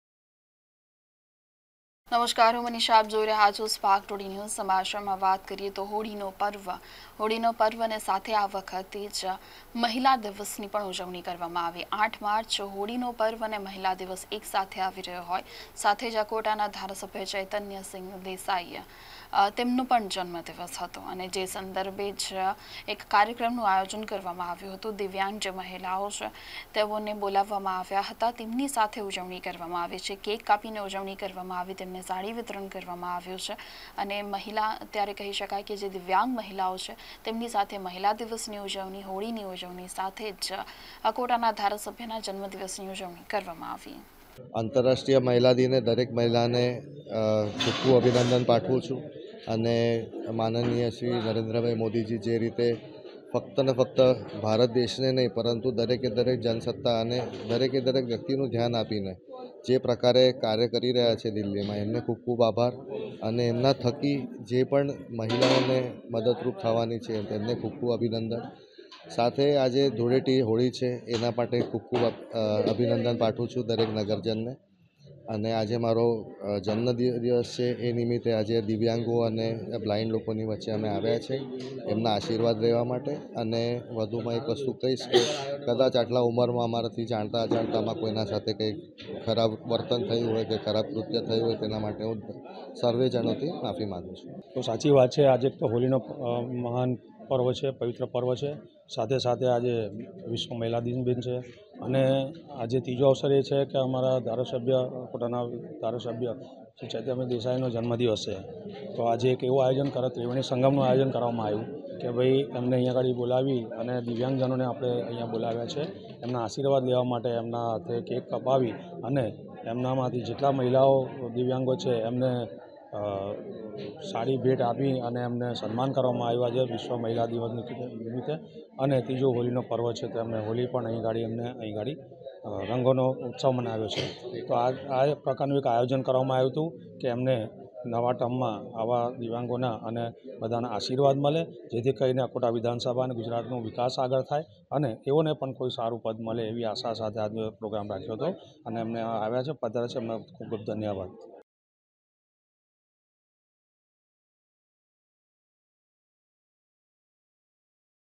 મહિલા દિવસ ધુળેટી અને અકોટા ધારાસભ્યના જન્મદિનની ઉજવણી 2023 | Spark Today News Vadodara
મહિલા દિવસ ધુળેટી અને અકોટા ધારાસભ્યના જન્મદિનની ઉજવણી